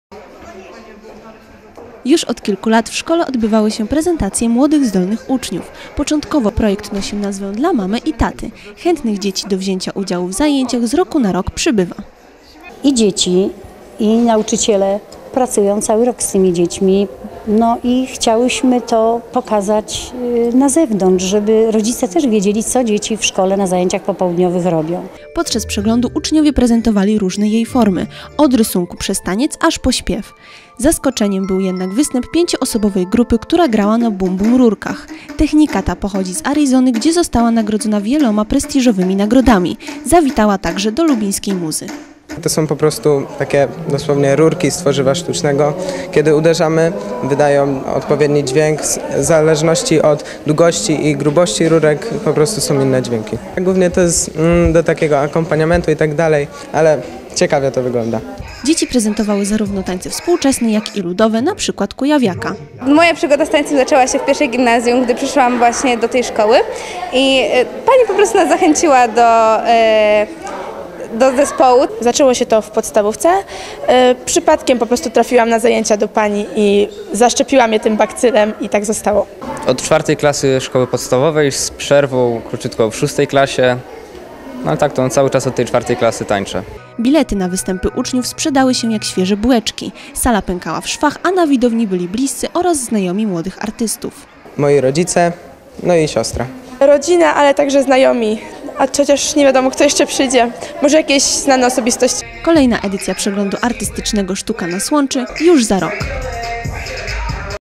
LUBIN. Połączyła ich sztuka
Taniec współczesny, breakdance, recytacja wierszy czy gra na „bum bum rurkach” - to wszystko w ramach akcji „Sztuka nas łączy” zorganizowanej przez Zespół Szkół Integracyjnych w Lubinie. Uczniowie prezentowali swoje talenty na scenie w Centrum Kultury „Muza” w Lubinie.